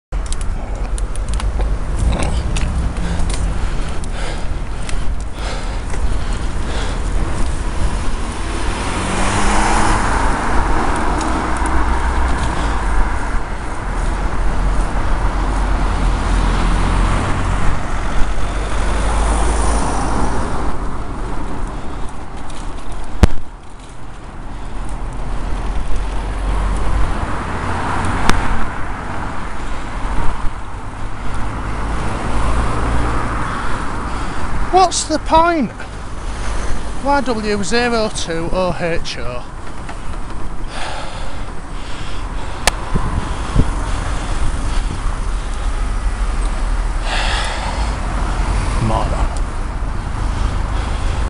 YW02OHO - Close, pointless pass
No need for this. Granted I was a bit slow off the line, but the range rover in front gave a decent berth, so why couldn't you? 8" from my handlebars and on the approach to a line of stationary traffic. Pointless and dangerous, especially seeing as I had time to filter to the front of traffic (and then some time for a breather) before the lights changed again.